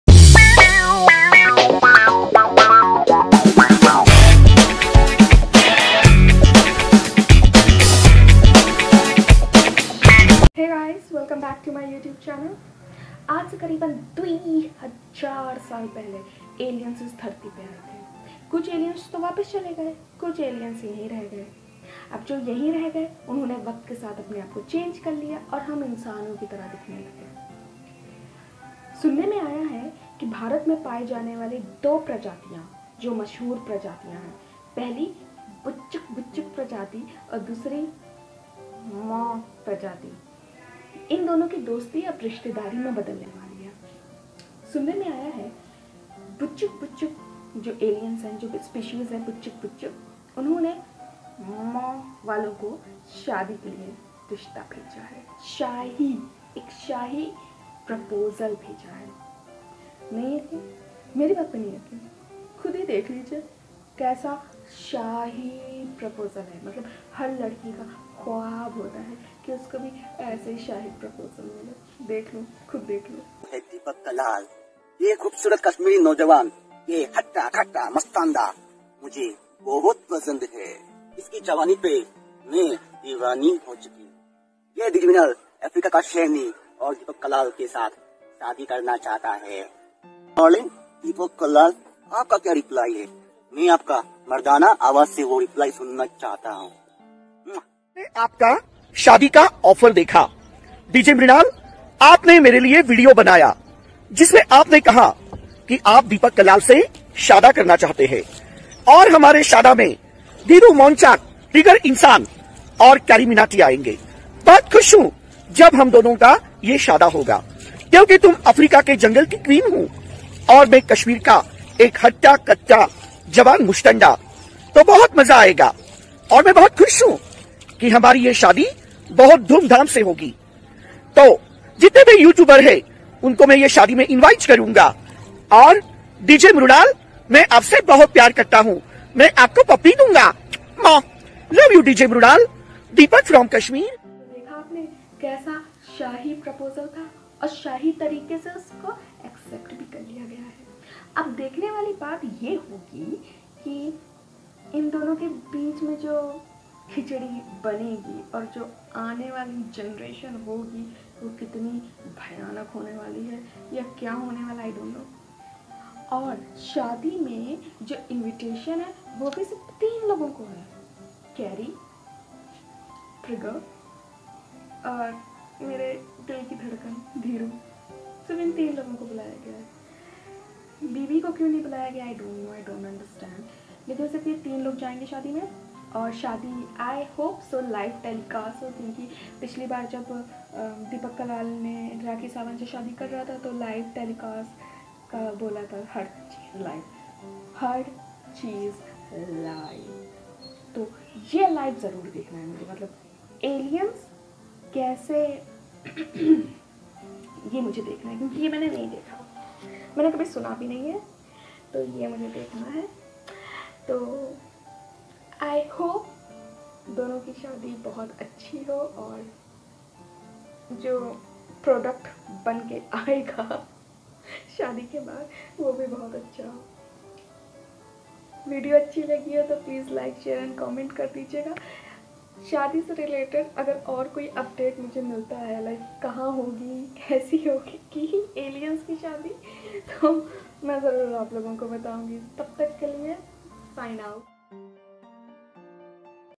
Perfect Couple || Best Jodi Ever || @dhirumonchik @Triggered Insaan Invited
Perfect Couple || Best Jodi Ever @dhirumonchik @trigger insan invited.

Hi Everyone,
(Unscripted Content)

This Video Is About:
DJ Mrinal and Deepak Kalal Ki anokhi shaadi and Shaadi ka grand proposal. Sabse anokhi shaadi me Dhirumonchik,Carryminati,aur Trigger Insan invited hai.

I hope you all like my content.

Afreen